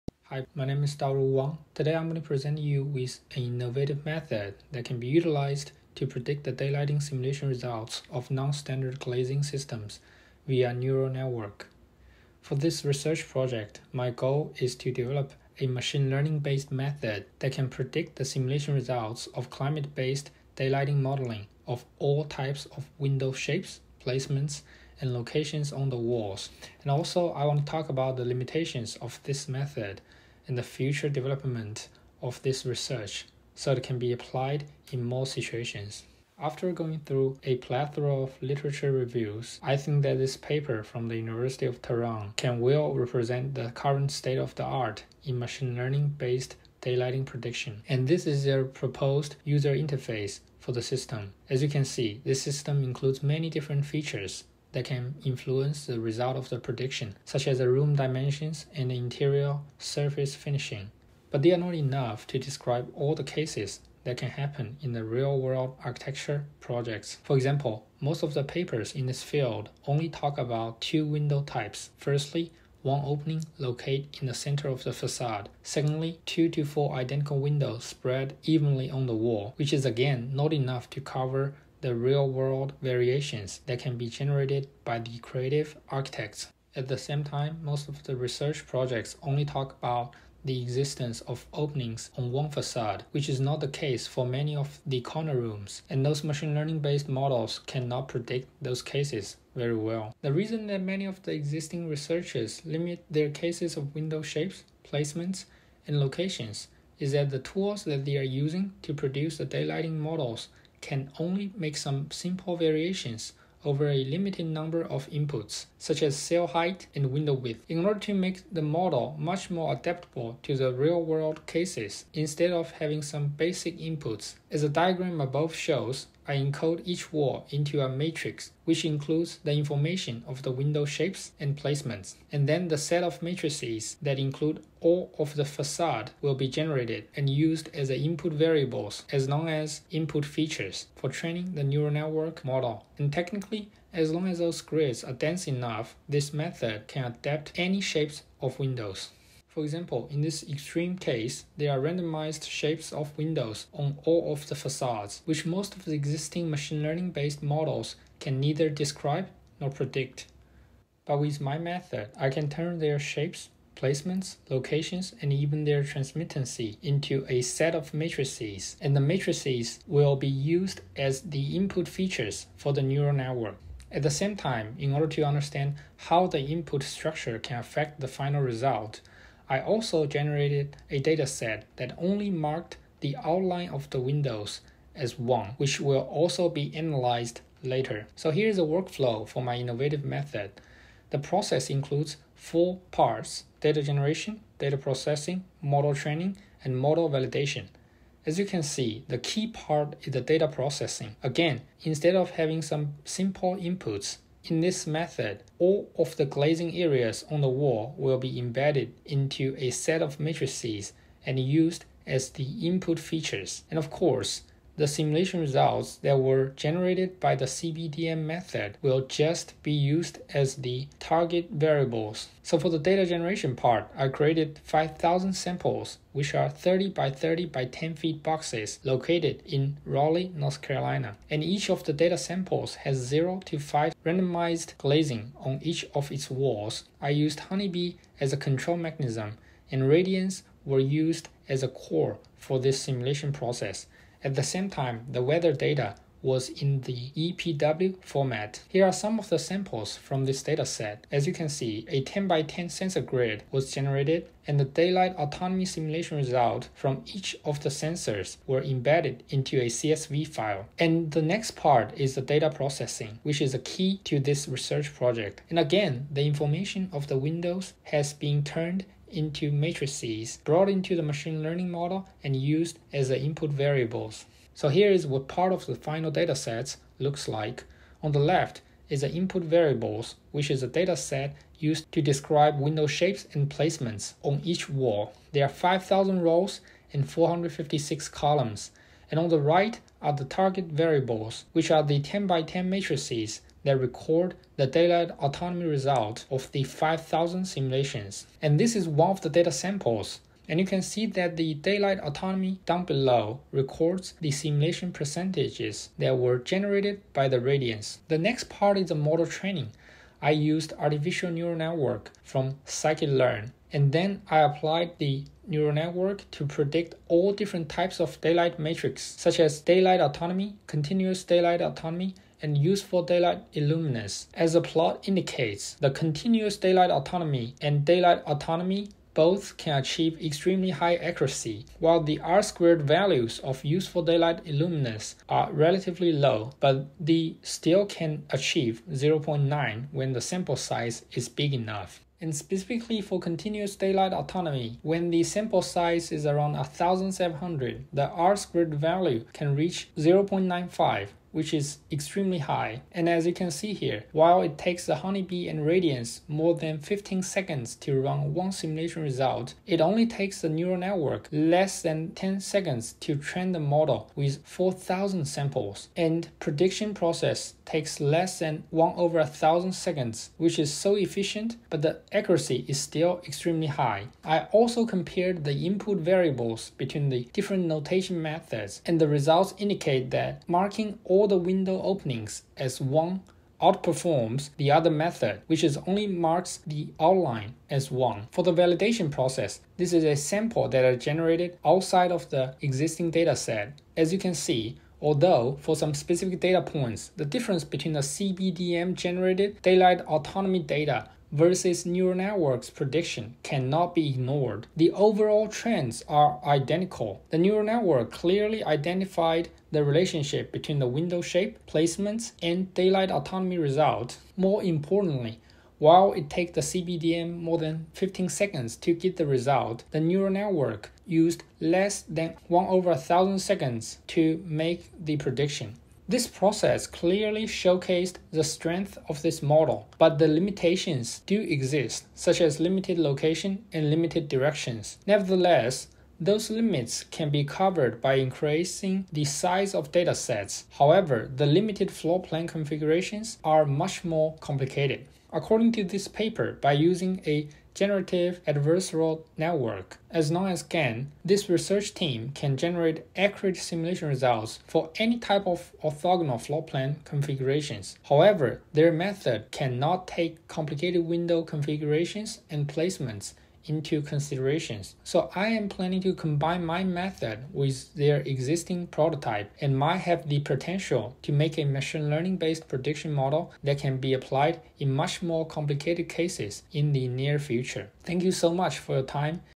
Daylight Simulation via Neural Network
This was my interest area presentation for my Ph.D. research. It was later published as a paper

Please read and consider citing it!